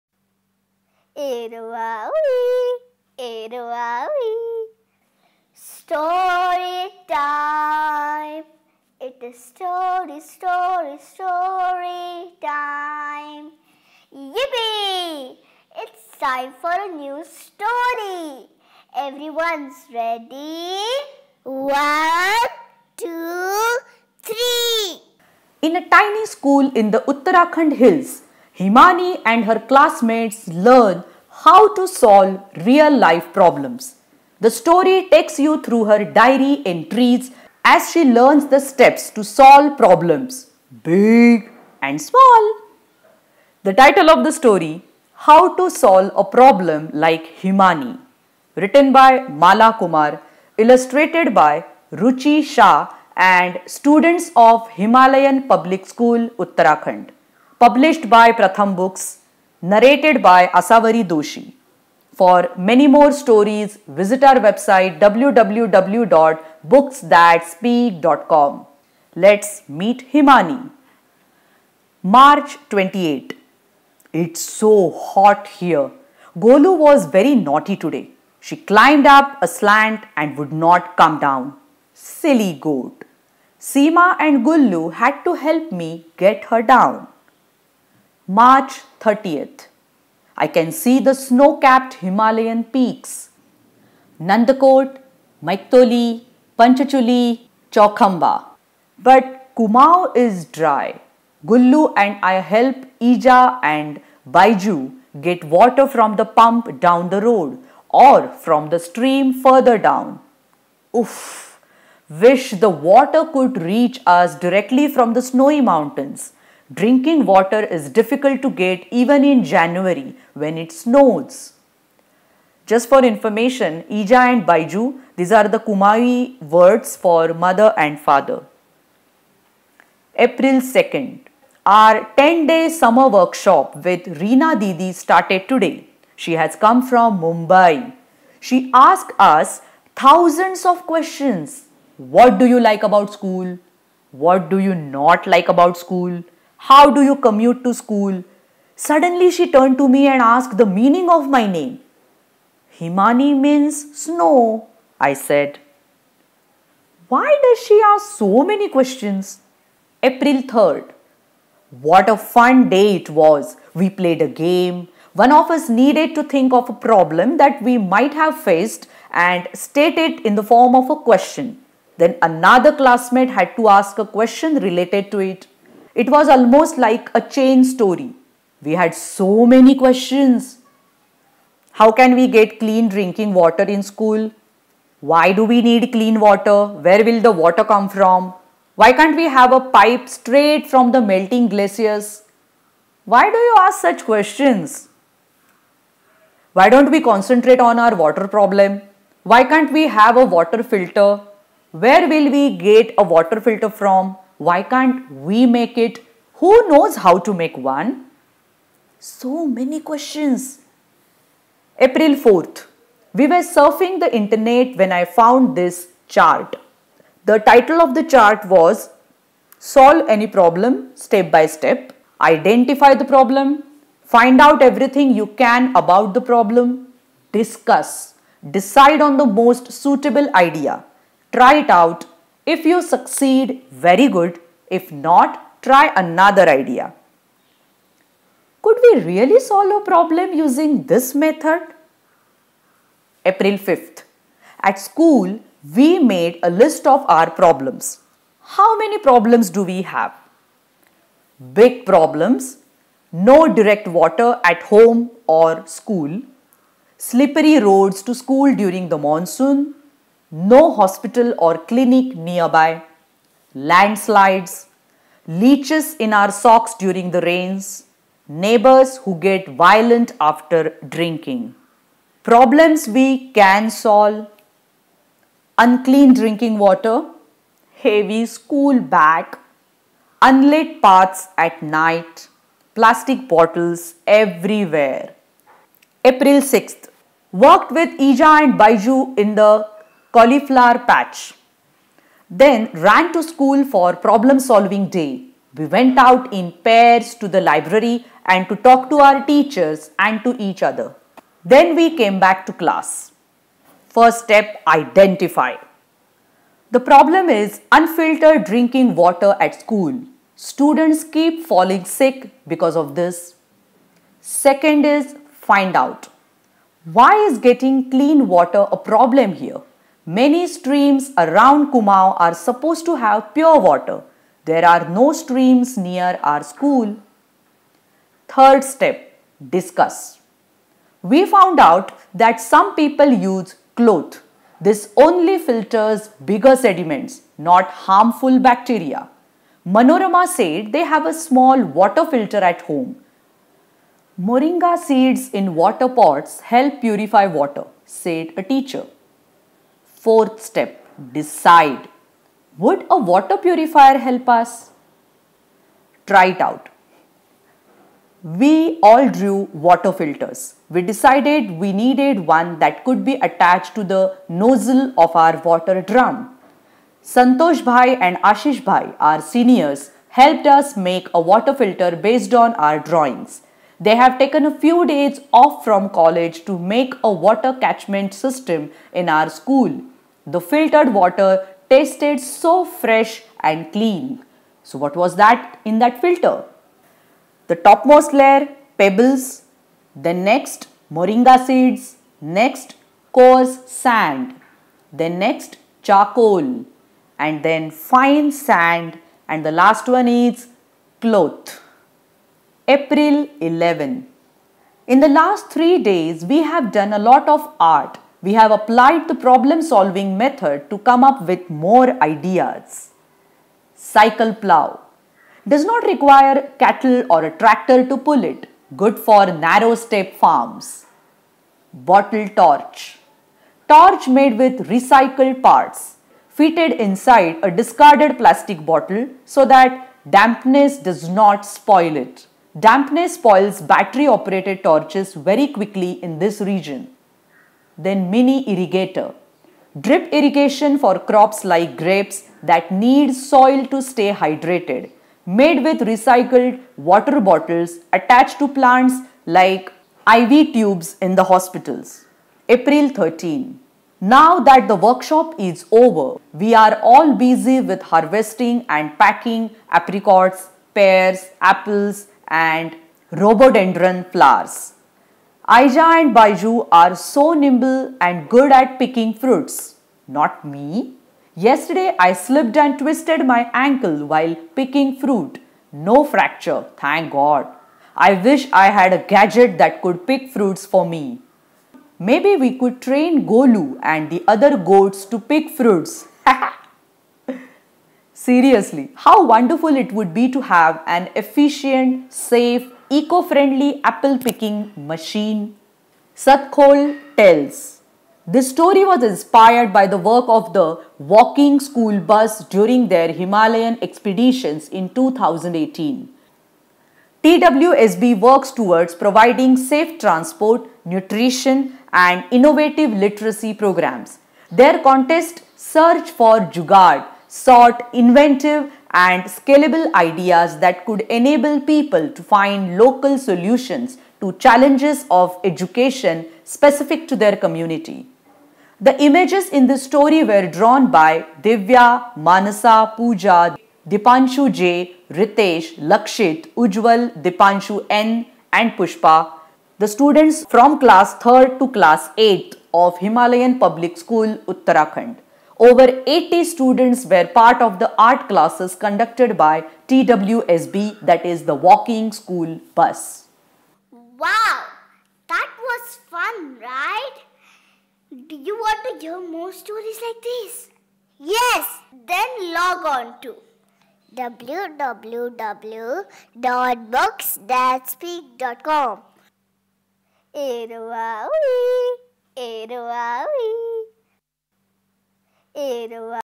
English Stories for Kids - How to Solve a Problem like Himani - Pratham Books
Written by Mala Kumar. Illustrated by Ruchi Shah and Students of Himalayan Public School.

In a tiny school in the Uttarakhand hills, Himani and her classmates learn how to solve real-life problems. The story takes you through her diary entries as she learns the steps to solve problems, big and small.